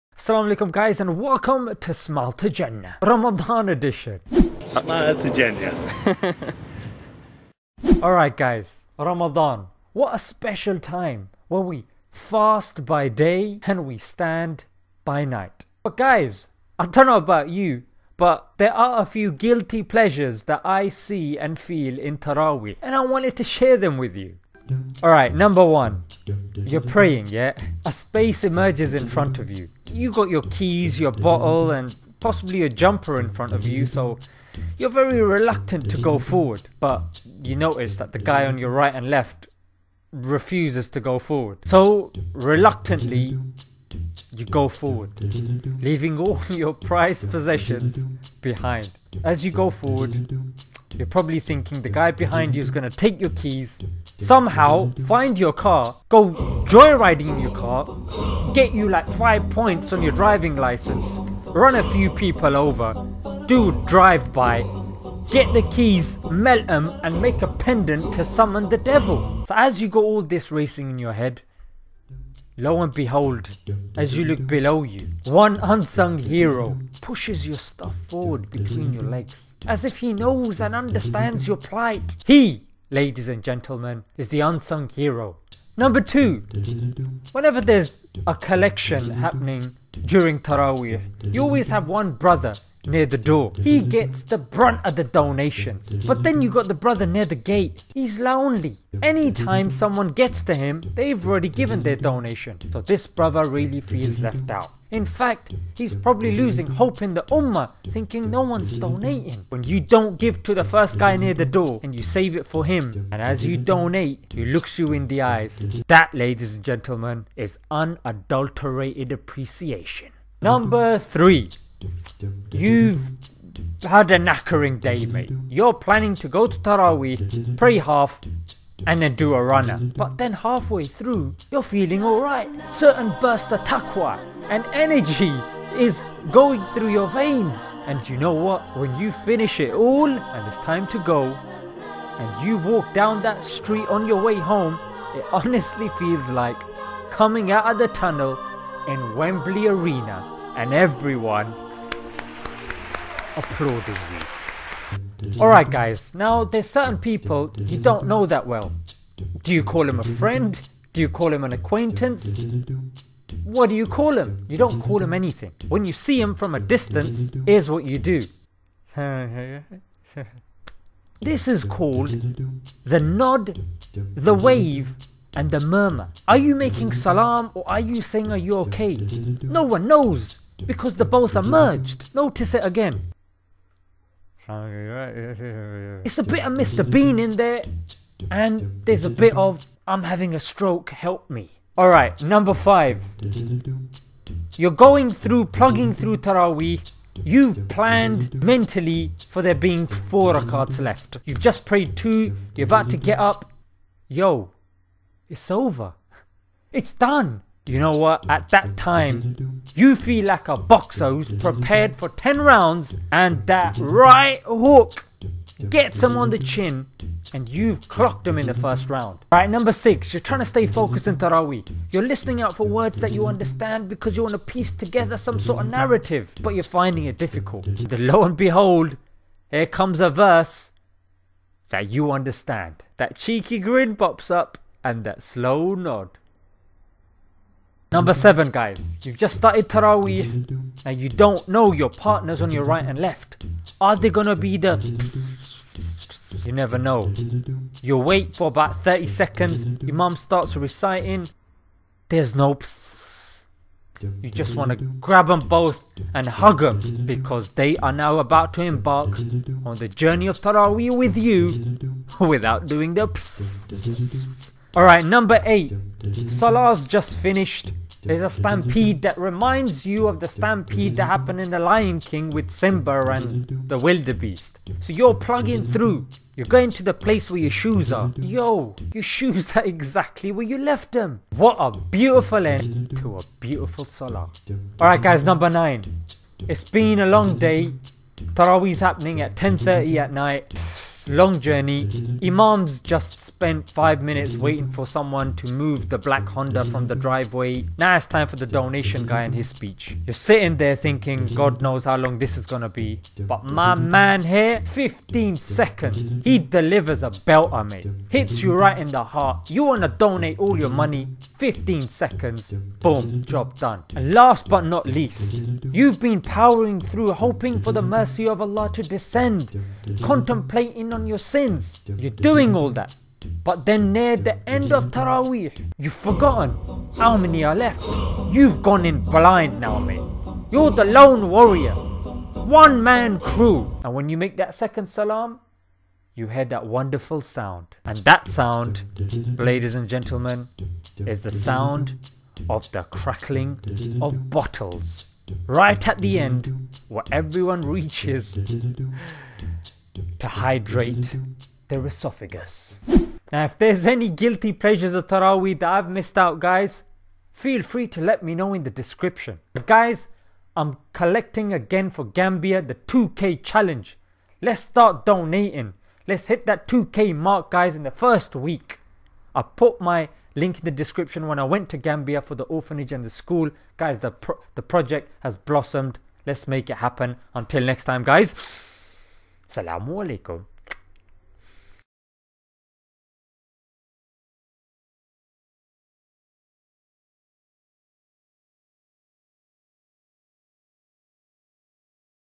GUILTY PLEASURES IN TARAWEEH
Here are some guilty pleasures I've experienced during Taraweeh, I'm sure you could relate to a few of these.